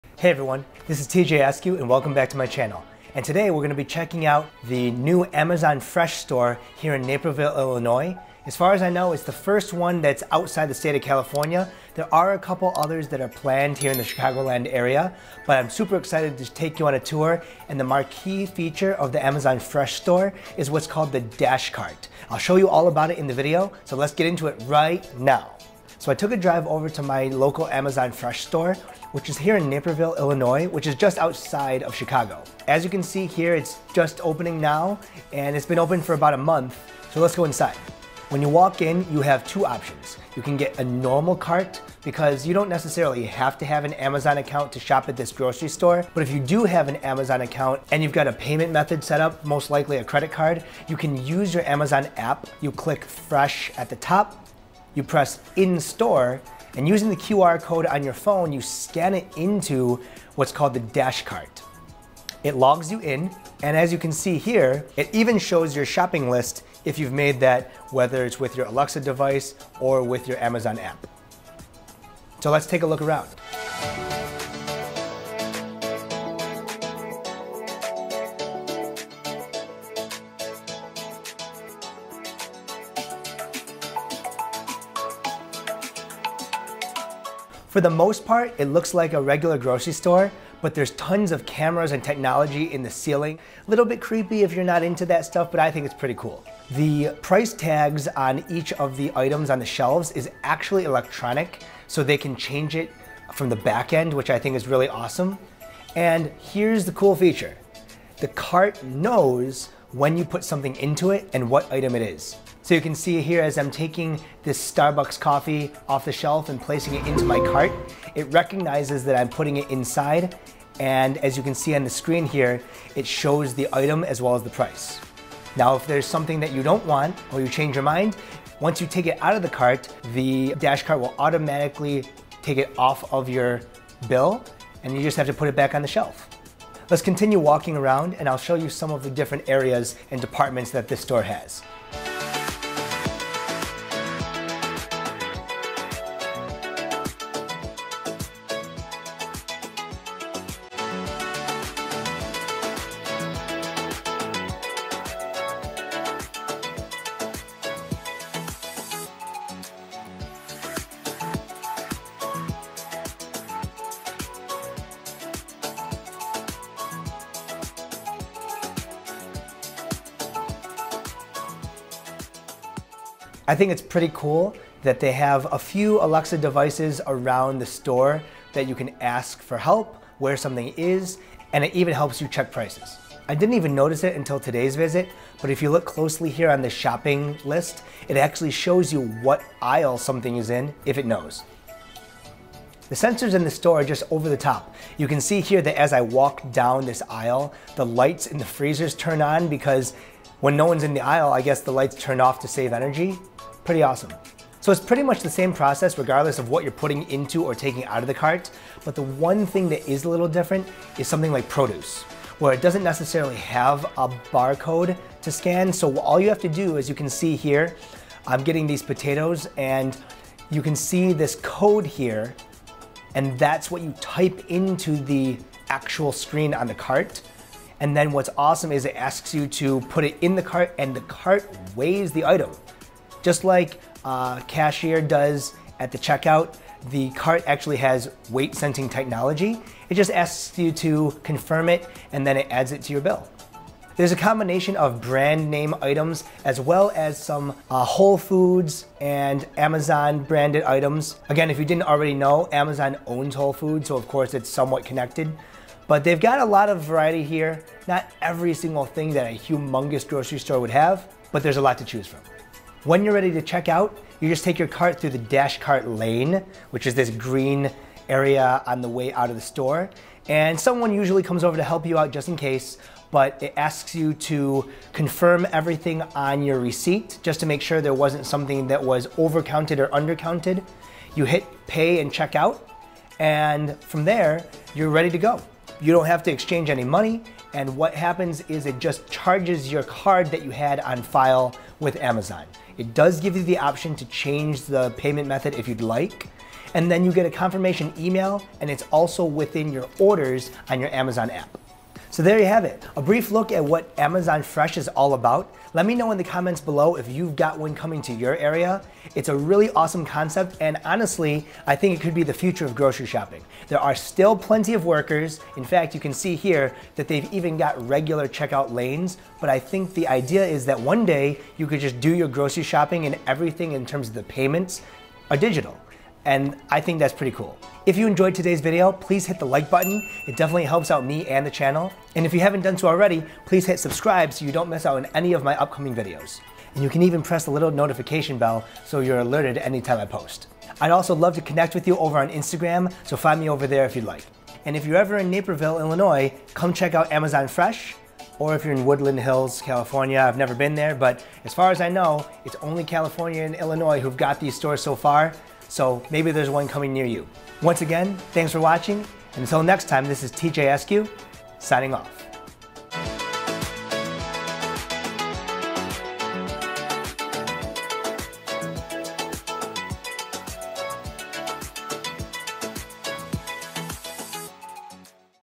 Amazon Fresh Grocery Store in NAPERVILLE, IL | Dash Cart is AWESOME!
Amazon is in the process of opening its own grocery stores called Amazon Fresh. These stores are different from the automated Amazon Go stores that have been popping up since 2019. The coolest part of these stores are the DASH CARTS that have tons of technology in them that scan your items as you put them into the cart, and that can also weigh your produce.

Check out the video for a look at what Amazon Fresh looks like. This location in Naperville, IL is one of the first ones that is outside the original pilot zone of southern California, like Woodland Hills. There are two more locations being built here in the Chicagoland area, so keep an eye out for one near you!